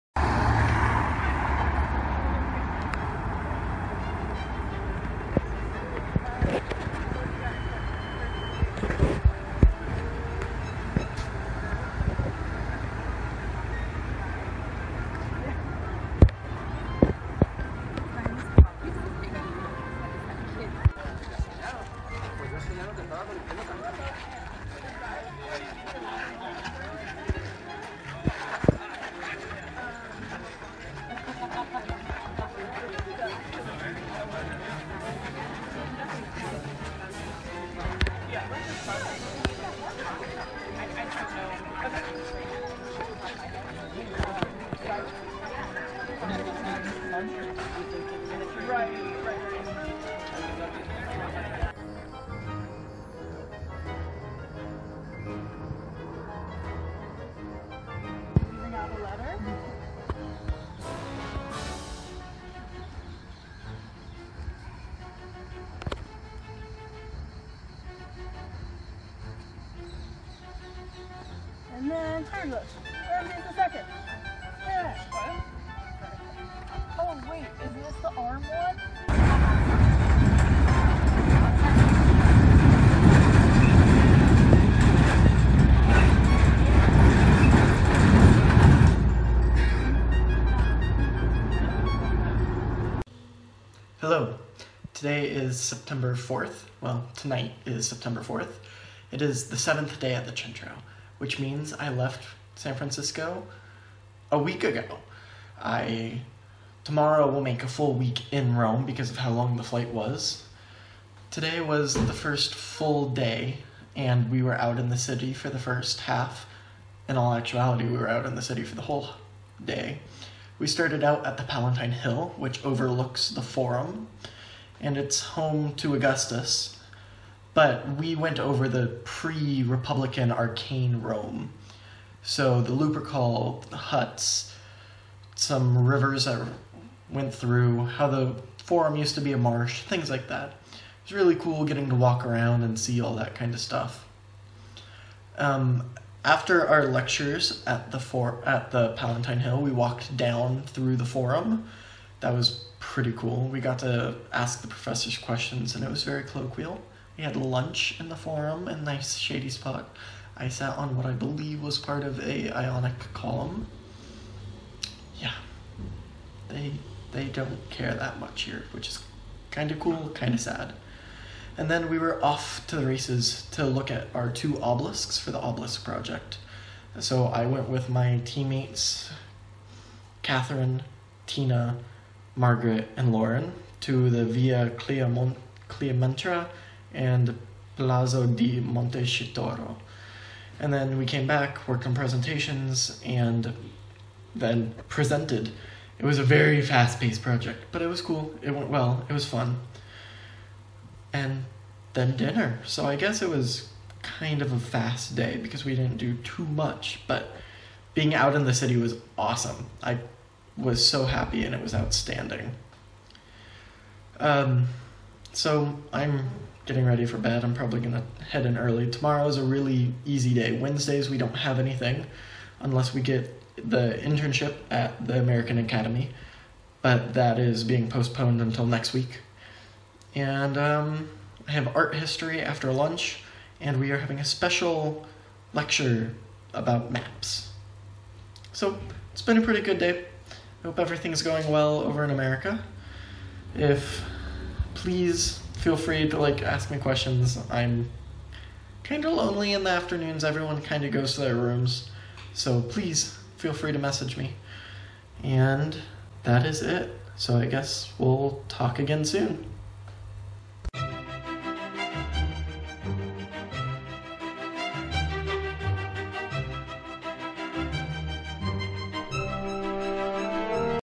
Palatine Hill and Some Obelisks 9-4-2018
First trip into the city. We visit the Palatine Hill and two Obelisks